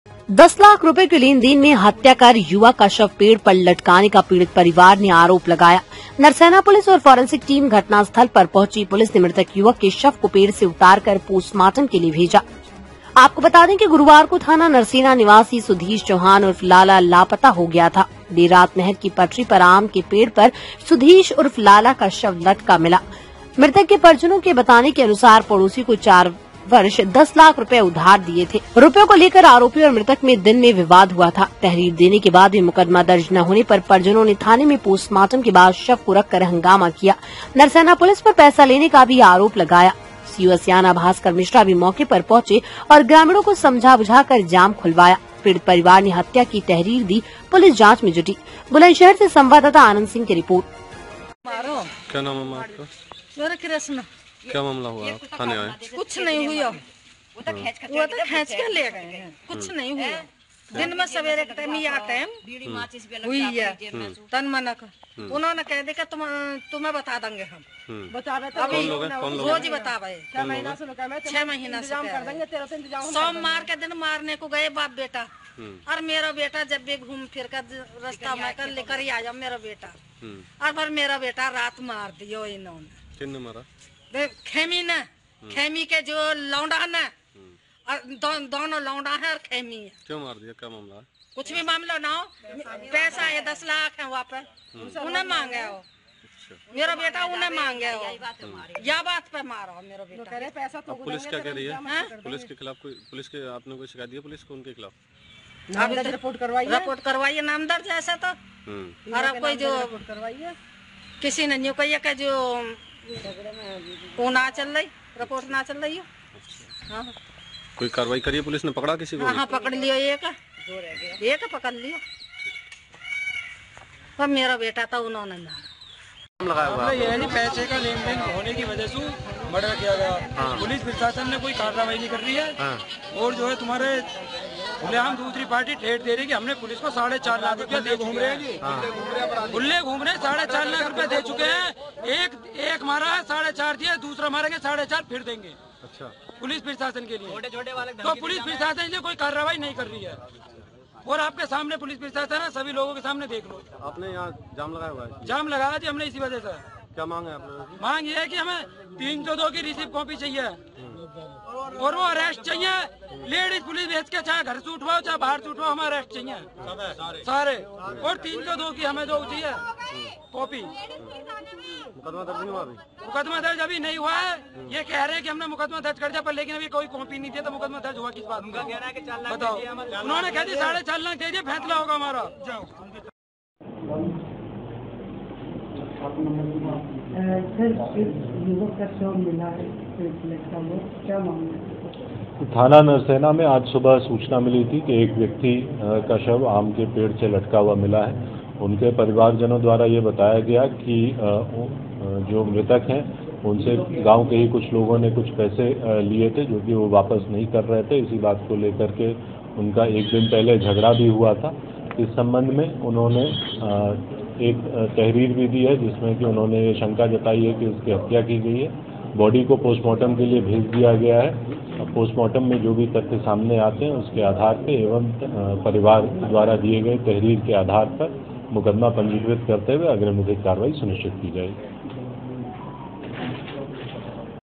GyamNewsIndia। हत्या ,आत्महत्या या साजिश ? मुकदमा दर्ज। परिजनों का रो रोकर बुरा हाल ॥
युवक की संदिग्ध परिस्थतियो मे मौत,युवक का शव पेड से लटका मिला

बुलंदशहर यूपी : दस लाख रुपयों के लेन-देन में हत्या कर युवक का शव पेड़ पर लटकाना का पीडित परिवार ने आरोप लगाया, नरसैना पुलिस और फोरेंसिक टीम घटना स्थल पर पहुची,पुलिस ने म्रतक युवक के शव को पेड से उतारकर पोस्टमार्टम के लिए भेजा,आपको बता दे कि गुरुवार को थाना नरसेना निवासी सुधीश चौहान उर्फ लाला लापता हो गया था,देर रात नहर की पटरी पर आम के पेड़ पर सुधीश उर्फ लाला का शव लटका शव मिला था,मृतक के परिजनो के बताये अनुसार पड़ोसी को 4 वर्ष 10 लाख रुपए उधार उधार दिये थे,रुपयों को लेकर आरोपी और मृतक में दिन में  विवाद हुआ था,तहरीर देने के बाद भी मुकदमा दर्ज न होने पर परिजनों ने थाने में पोस्टमार्टम के बाद शव को  रखकर हंगामा किया नरसैना पुलिस पर पैसा लेने का भी आरोप लगाया ,सीओ स्याना भास्कर मिश्रा भी मौके पर पहुचे और ग्रामीणो को समझा बुझाकर जाम खुलवाया पीडित परिवार ने हत्या की तहरीर दी,पुलिस जांच मे जुटी।

बाइट =श्लोक कुमार एसएसपी

आनन्द सिंह संवाददाता बुलंदशहर